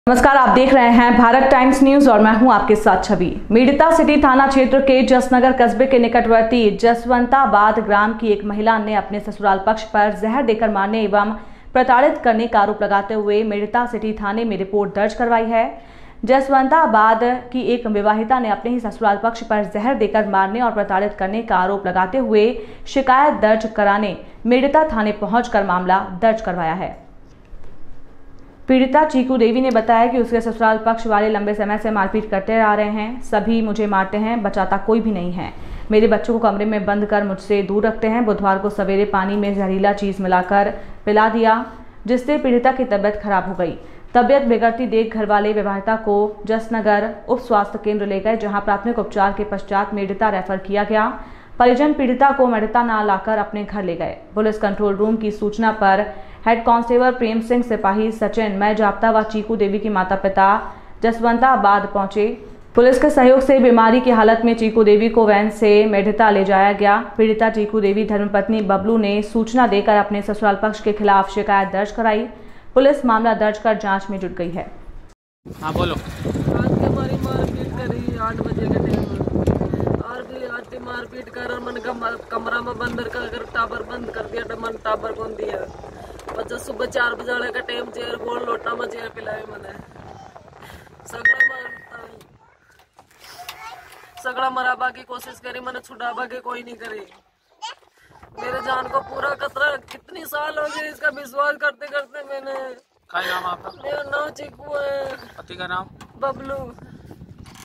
जसवंताबाद ग्राम की एक महिला ने अपने सुसराल पक्ष पर जहर देकर मारने का लगाया आरोप,
मेड़ता सिटी पुलिस जुटी जांच में

मेड़ता रोड
एजाज़ अहमद उस्मानी

मेड़ता सिटी थाना क्षेत्र के जसनगर  कस्बे के निकटवर्ती जसवंताबाद ग्राम की एक महिला ने अपने सुसराल पक्ष पर जहर देकर मारने एवं प्रताड़ीत करने का आरोप लगाते हुए  मेडतासिटी थाने  में रिपोर्ट दर्ज  करवाई है। जसवंताबाद ग्राम की एक विवाहिता ने अपने ही सुसराल पक्ष पर जहर देकर मारने एवं प्रताड़ीत करने का आरोप लगाते हुए शिकायत दर्ज कराने मेडता थाने  में पहुंच कर मामला दर्ज करवाया । पीड़ीता चीकू देवी ने बताया कि उसके सुसराल पक्ष वाले लंबे समय से मारपीट करते आ रहे है। सभी मुझे मारते है, बचाता कोई भी नही है ।  मेरे बच्चों को कमरे में बंद कर मुझसे दूर रखते है। बुधवार को सवेरे  पानी में जहरीली चीज मिलाकर पिला दी   जिससे पीडिता की तबियत खराब हो गई।  तबीयत बिगड़ती देख घरवाले विवाहिता को जसनगर उप स्वास्थ्य केंद्र ले गए  । जहां प्राथमिक उपचार के पश्चात मेडता रेफर कर दिया गया ।  परिजन पीड़ीता को मेडता ना लाकर अपने घर ले गए ।  पुलिस कंटौल रूप की सूचना पर हैड कास्टेबल प्रेमसिंह, सिपाही सचिन मय जाब्ता व चीकू देवी की माता सीता  जसवंताबाद  पहॅुची । पुलिस के सहयोग से बीमारी की हालत में चीकू देवी  को वैन से मेडता ले जाया गया । पीड़ीता  चीकू देवी धर्मपत्नी बबलू ने सूचना देकर अपने ससुराल पक्ष के खिलाफ शिकायत  दर्ज करवाई । पुलिस मामला दर्ज कर जांच में जुट गई है।